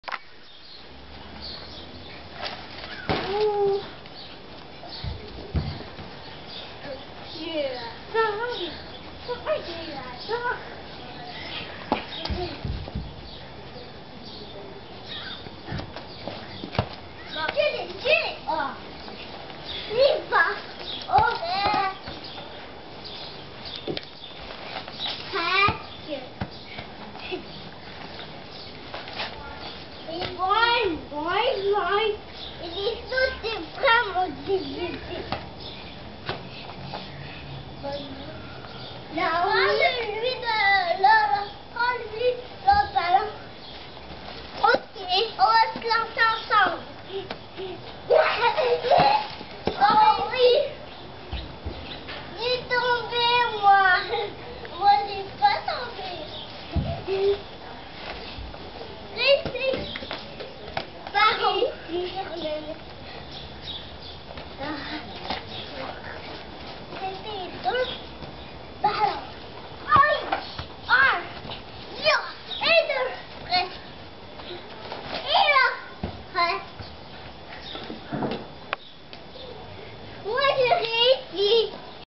Preko ograde
Disha i Naomi prebacuju loptu preko (zive) ograde